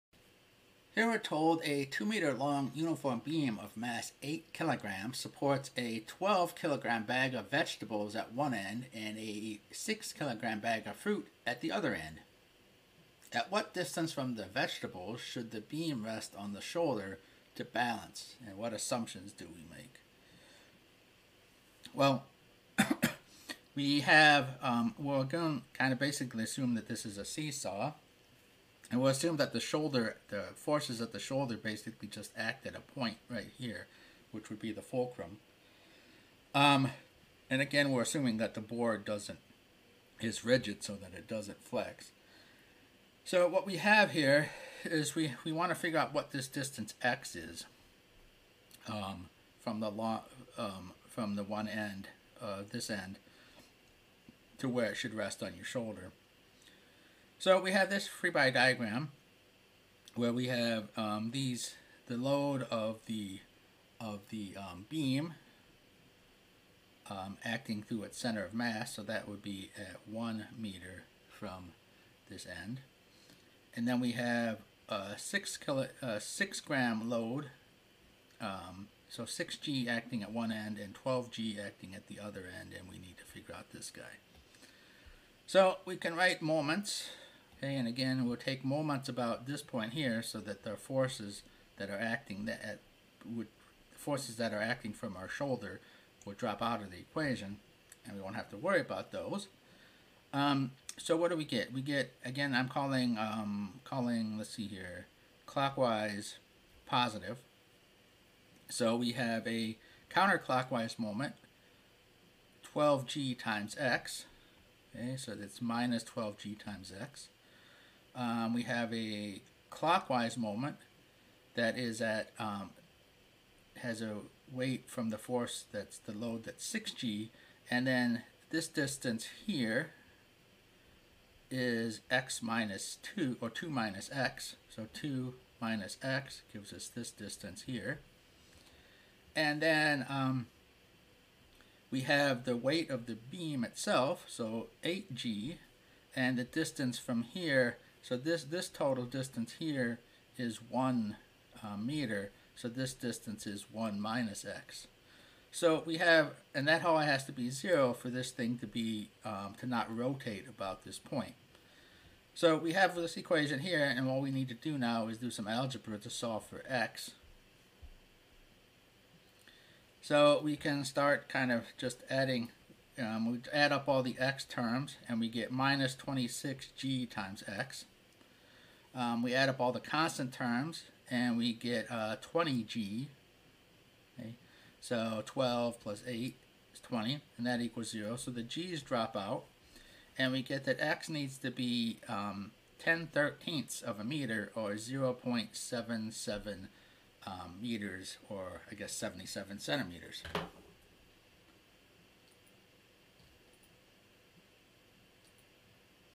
A 2 0 m long uniform beam of mass 8 0 supports a bag of vegetables at one end and a bag of fruit at
A 2.0 -m-long uniform beam of mass 8.0
 supports a
 bag of vegetables at one end and a
 bag of fruit at the other end. At what distance from the vegetables should the beam rest on your shoulder to balance? What assumptions did you make?